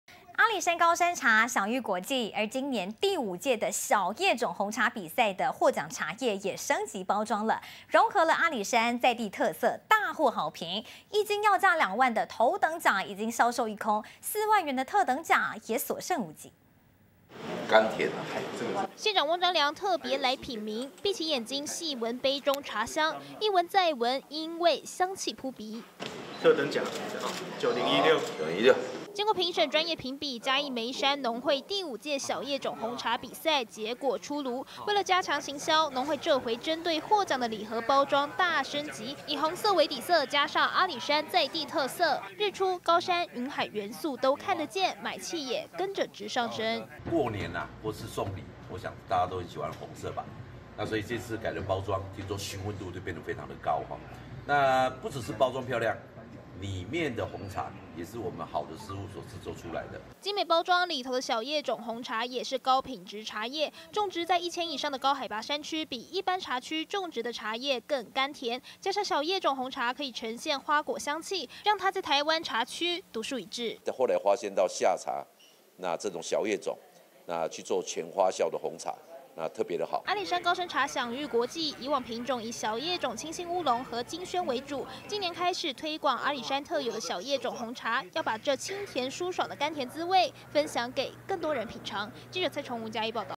日出.高山.雲海通通有 大紅包裝獲好評
阿里山高山茶享譽國際，今年第五屆小葉種紅茶比賽的獲獎茶葉，也升級包裝，融合阿里山在地特色，大獲好評，一斤要價2萬的頭等獎已經銷售一空，4萬元的特等獎也所剩無幾。